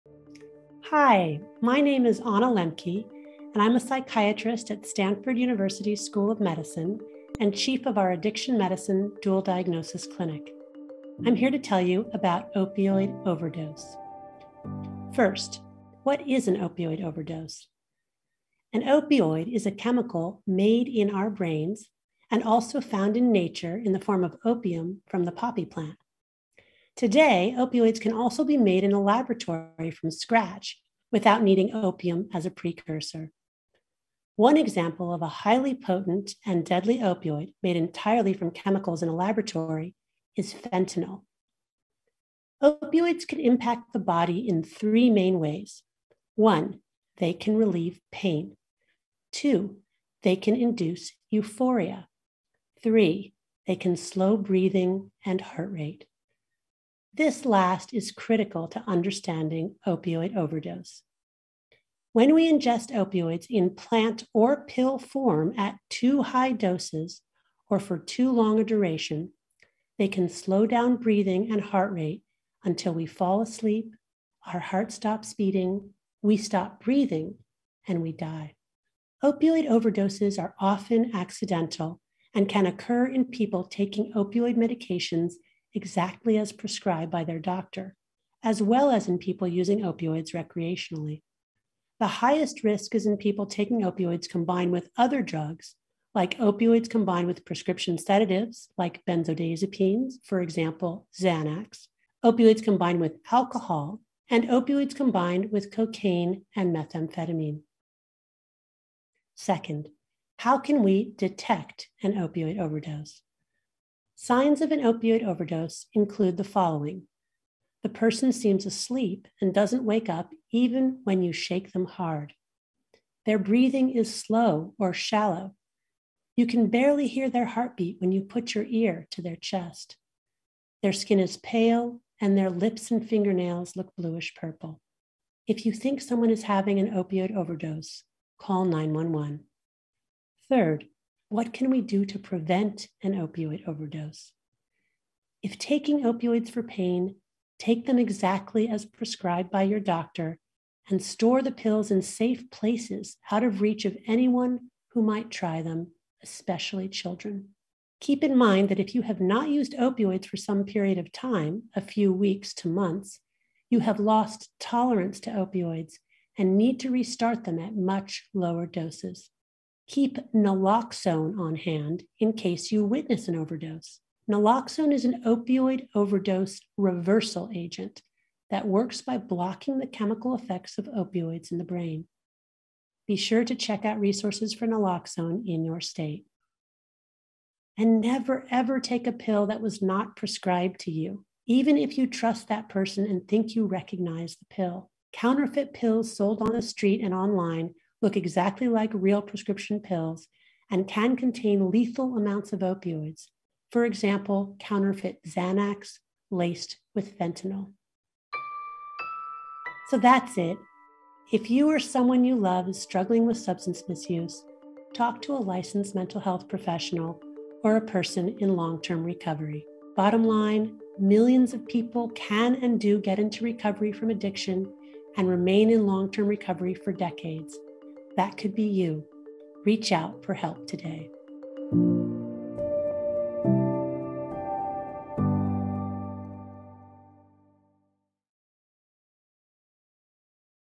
Opioid Overdose: Dr. Anna Lembke explains the signs & prevention
In this video, Dr. Anna Lembke discusses how to recognize opioid overdoses. She explains how opioids affect the body, including how they slow breathing and heart rate, sometimes to life threatening levels. Opioid overdoses are often accidental, and it’s critical to know the signs and how to prevent them from happening.

0:00 Introduction
00:16 What is an opioid overdose?
1:50 Signs of an opioid overdose
2:20 What can we do to prevent an opioid overdose?
3:30 Reach out for help

Dr. Anna Lembke is an addiction medicine specialist and chief of the addiction medicine dual diagnosis clinic at Stanford Medicine.

This video is a production of the Stanford Center for Health Education team, in collaboration with Stanford Medicine and the Stanford Center for Professional Development.